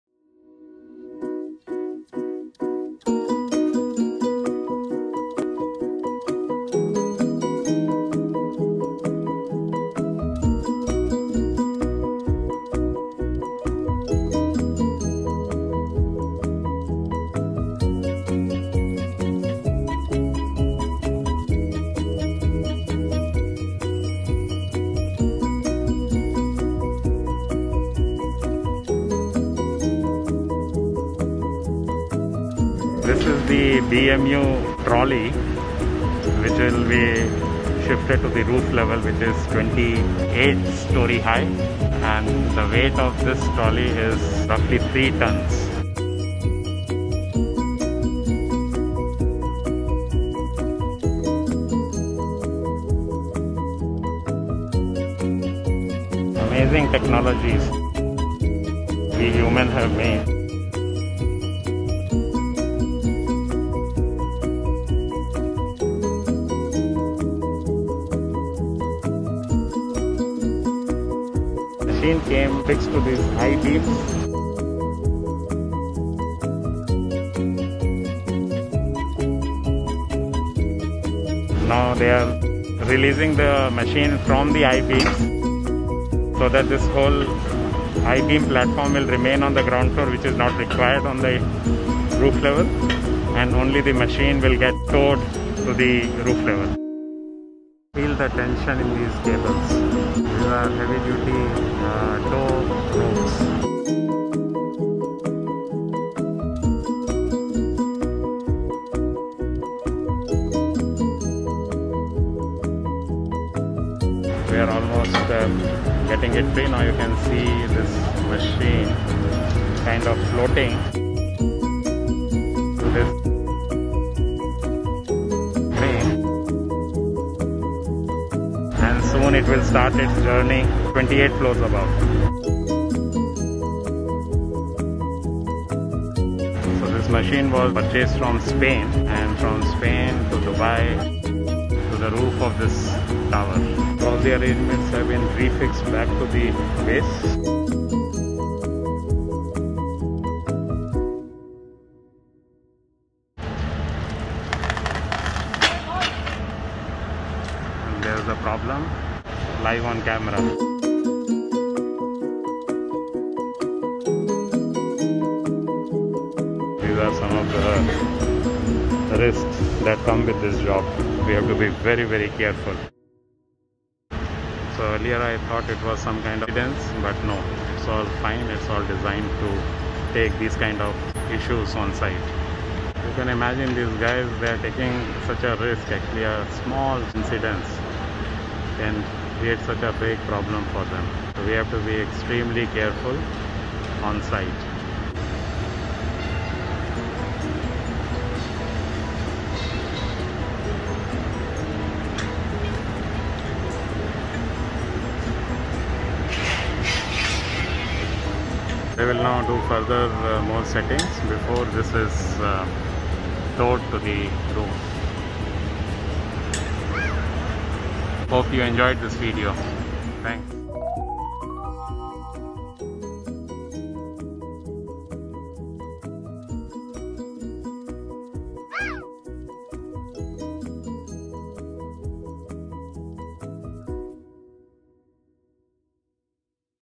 HOW TO TRANSFER BMU TROLLEY TO ROOF OF A SKYSCRAPER PART-1
Every this you want to know about a a 3 ton BMU trolley is transferred to a 28 story high Skyscraper and what are the risks involved in this task.